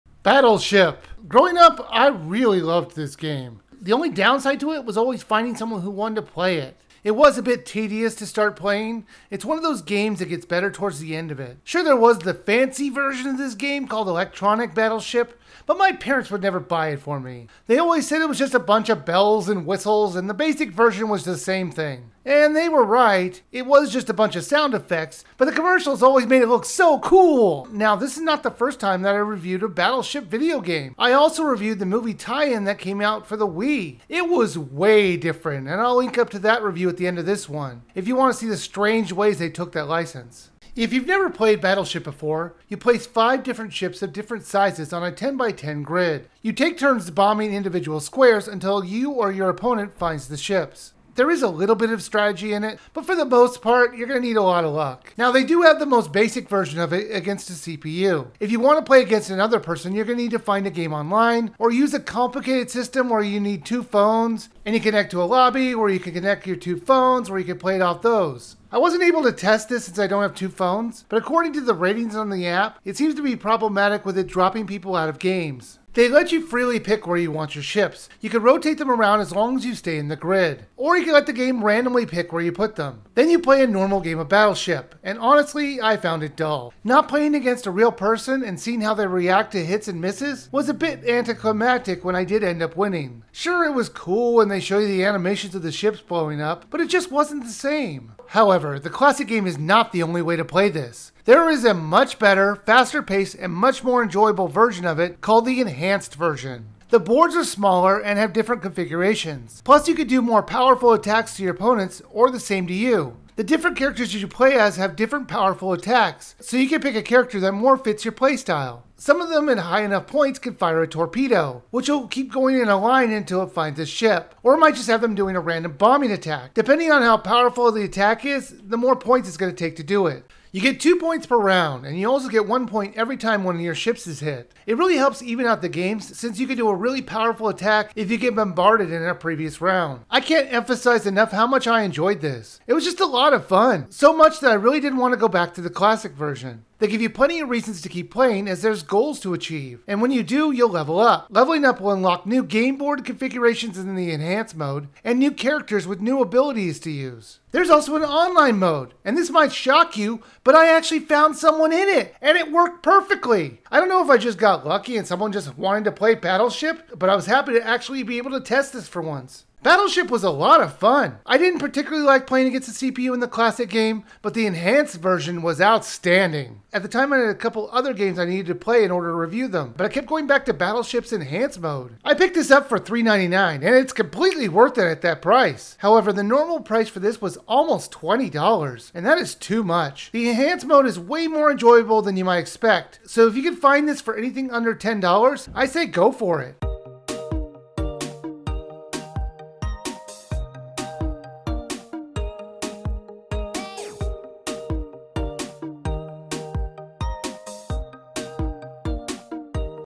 Battleship Review (Nintendo Switch)
The Wiiviewer reviews the better then expected Battleship game for the Switch!